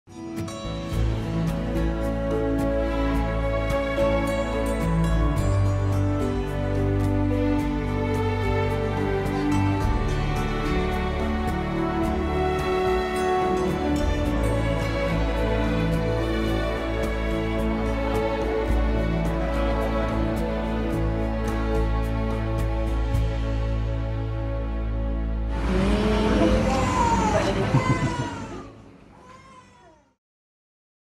Ipoh Hospital welcomes 10 Merdeka babies
As the nation celebrates National Day, new arrivals brought much happiness to many couples as eight girls and two boys were born on National Day as of 4.30pm at the Raja Permaisuri Bainun Hospital in Ipoh, Perak.